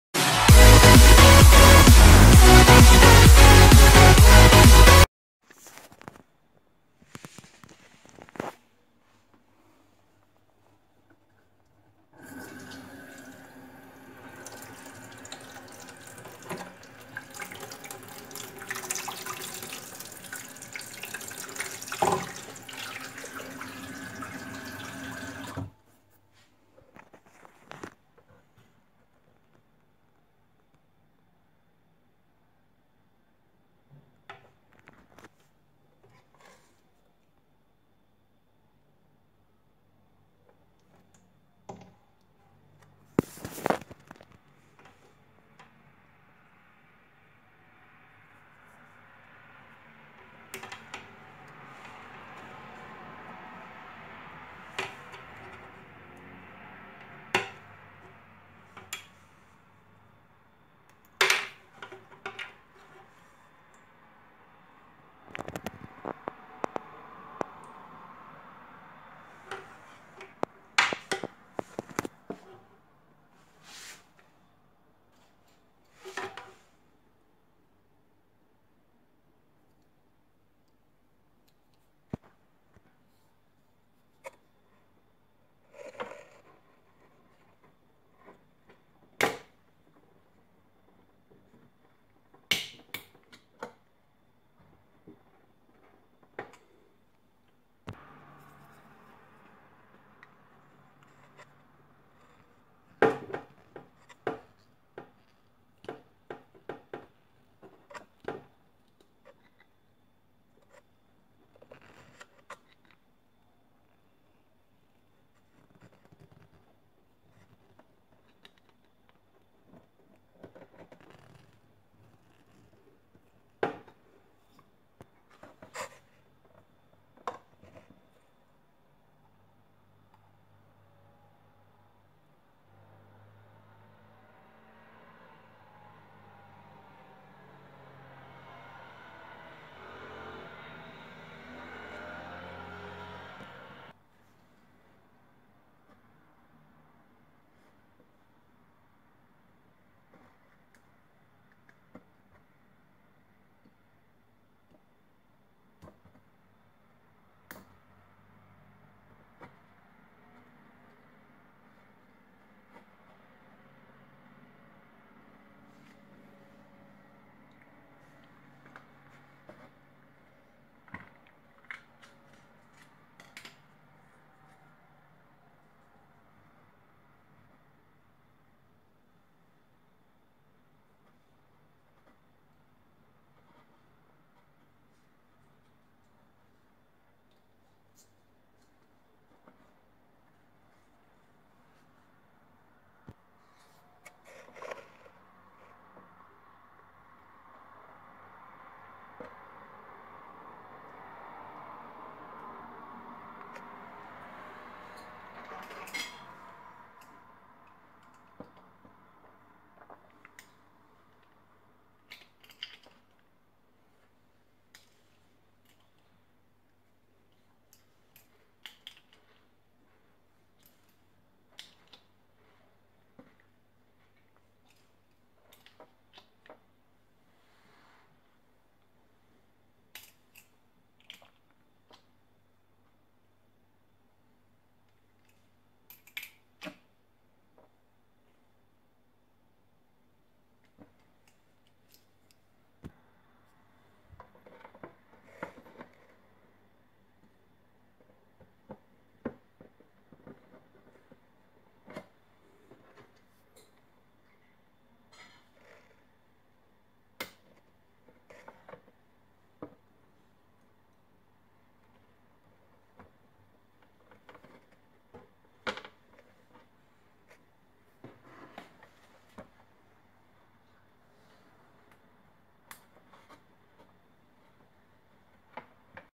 RO su kaçağı sorunu çözüldü
su arıtma cihazının regülatörüne bağlanan hortumların tutucu rekorun çatlamasıyla su kaçağına sebep olan parça değiştirilerek sorun giderildi